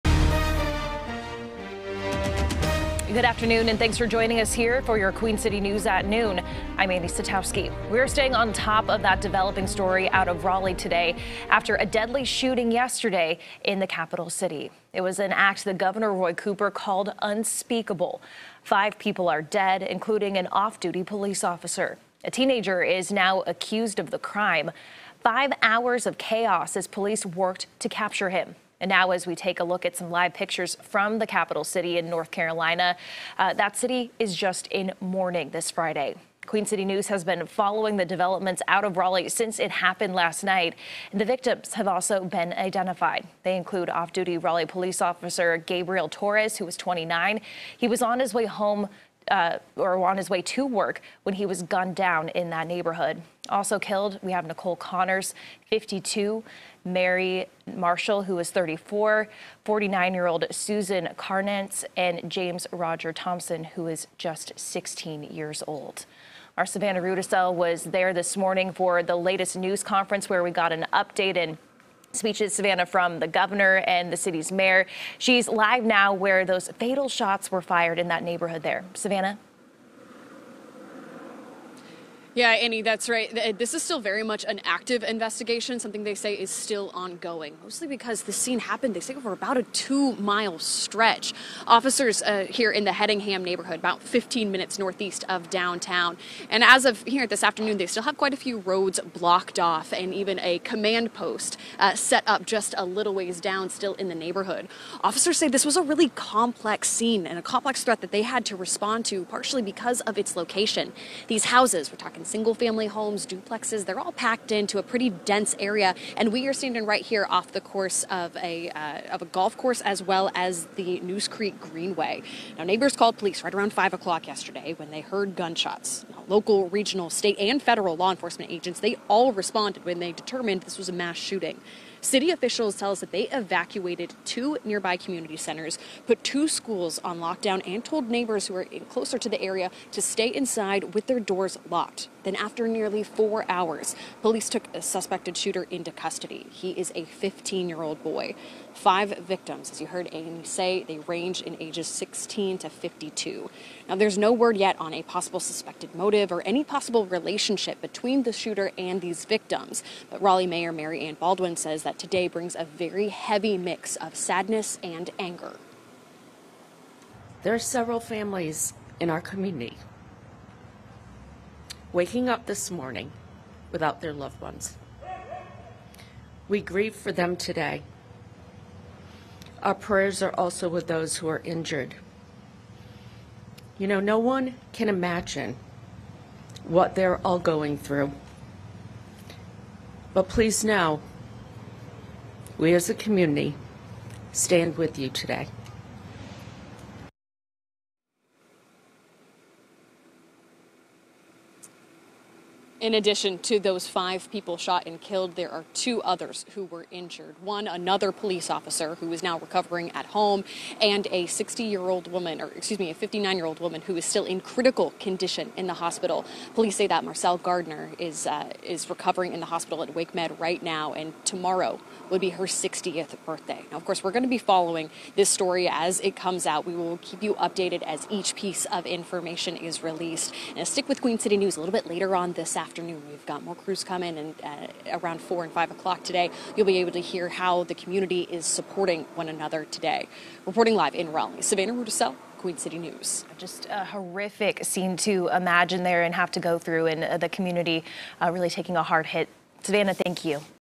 Police chief names victims of Raleigh mass shooting; 5 dead, 2 injured
Raleigh Police Chief Estella Patterson, Raleigh Mayor Mary-Ann Baldwin and Gov. Roy Cooper gathered Friday to share new details about the mass shooting that killed five and injured two in Raleigh on Thursday. Patterson identified the five victims killed, including off-duty police officer Gabriel Torres, 29, who was on his way to work when he was gunned down.

Also killed were Nicole Conners, 52, Mary Marshall, 34, Susan Karnatz, 49, and James Roger Thompson, 16.

QCNEWS.COM